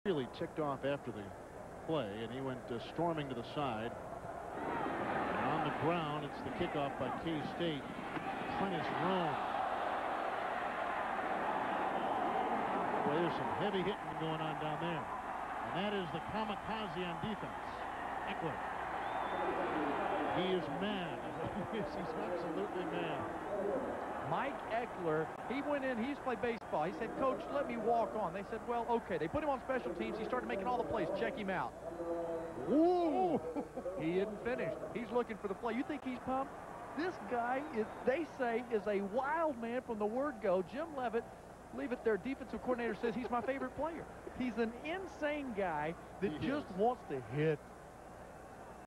Mike Ekeler Copper Bowl
Mike Ekeler on special teams during the 1993 Copper Bowl.